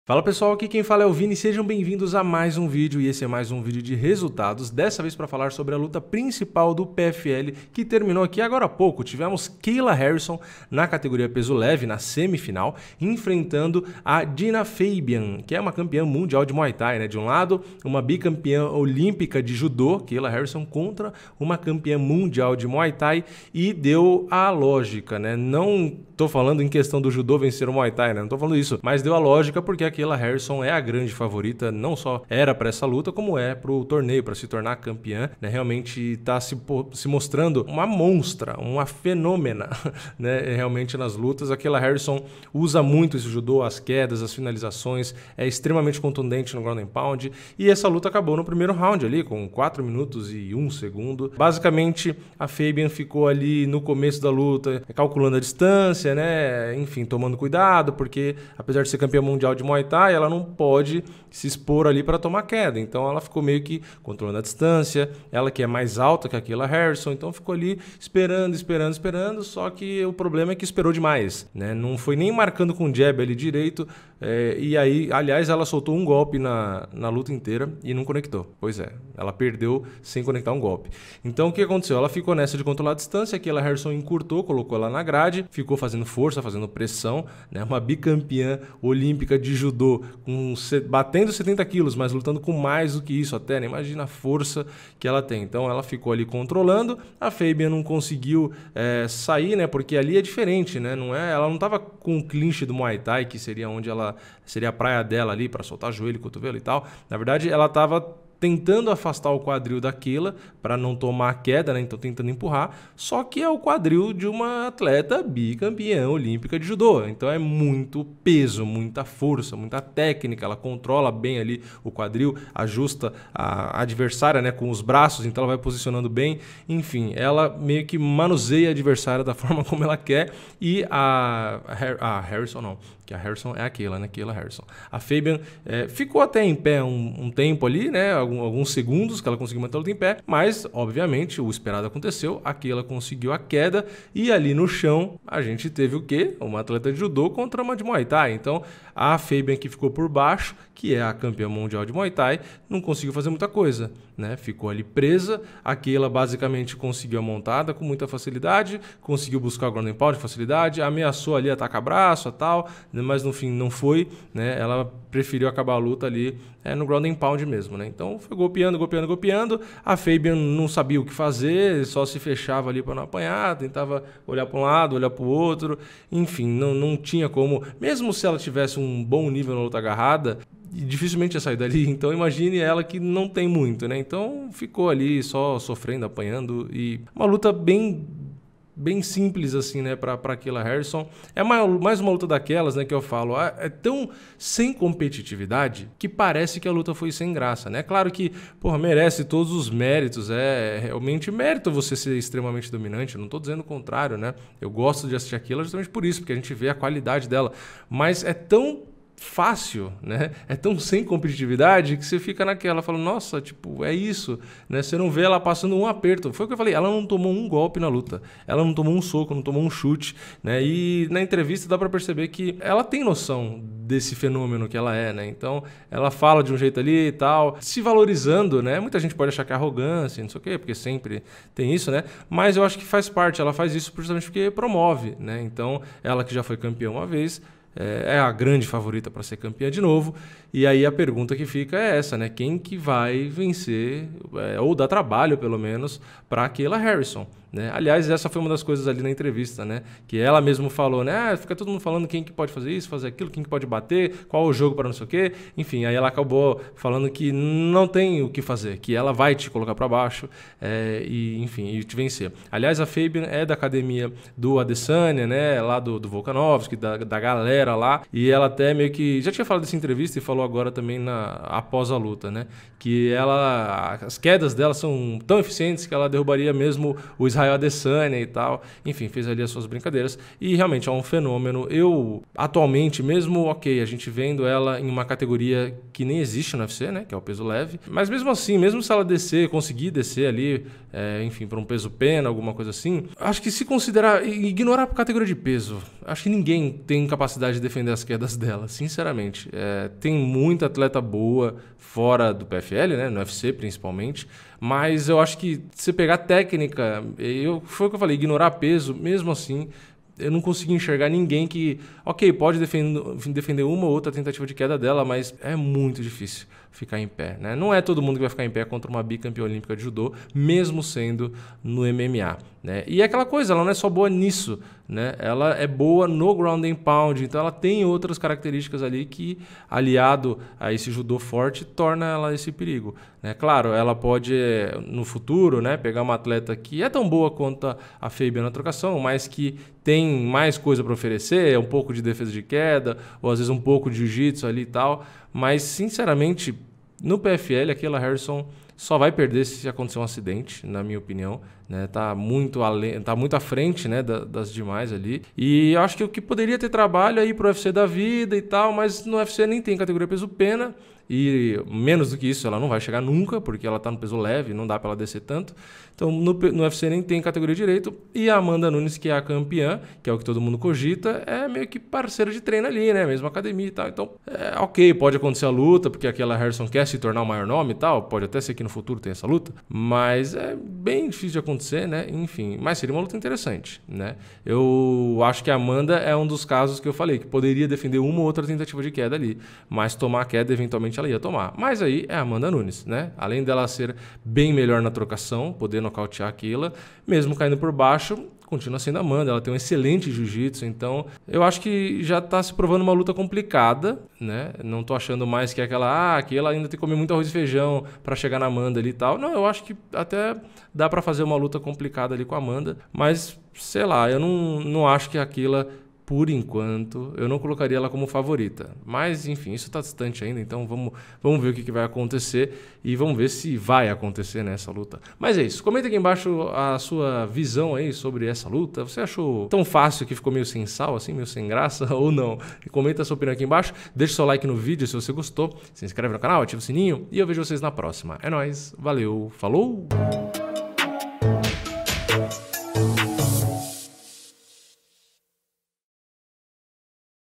KAYLA HARRISON ATROPELOU MAIS UMA VEZ! RESULTADO PFL
Resultado e comentários da luta entre Kayla Harrison e Genah Fabian que rolou no PFL.